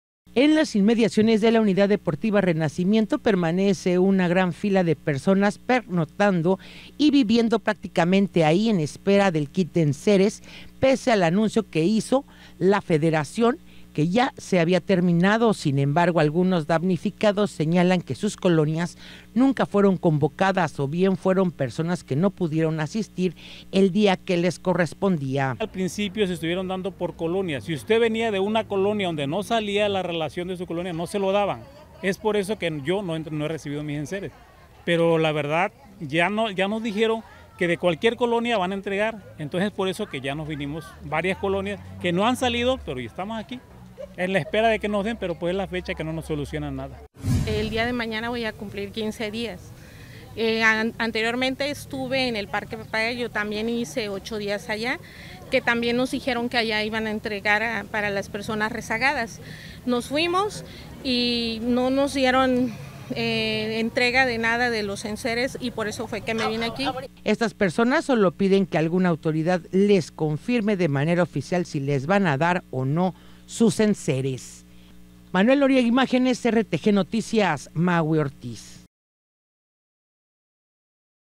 RTGnoticias | Damnificados con cintillo no se moverán de UDA Renacimiento
En las inmediaciones de la unidad deportiva renacimiento permanece una gran fila de personas pernoctando y viviendo prácticamente ahí en espera del kit de enseres, pese al anuncio que se hizo por parte de la federación que ya se había terminado.